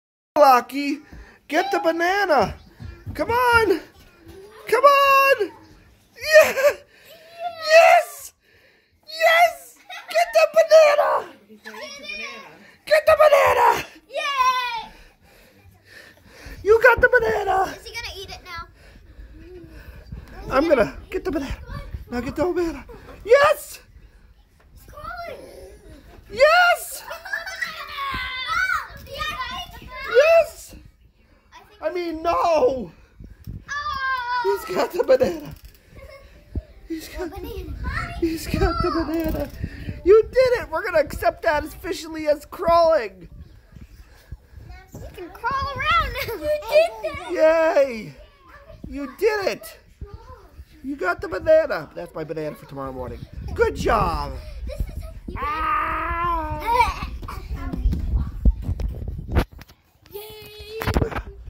1,190 - Lachlan Officially Crawls for the First Time!
It's official, at about 10-11 months old, Lachlan can crawl! (He got the banana! lol) He will probably be walking soon like all of his brothers did. ...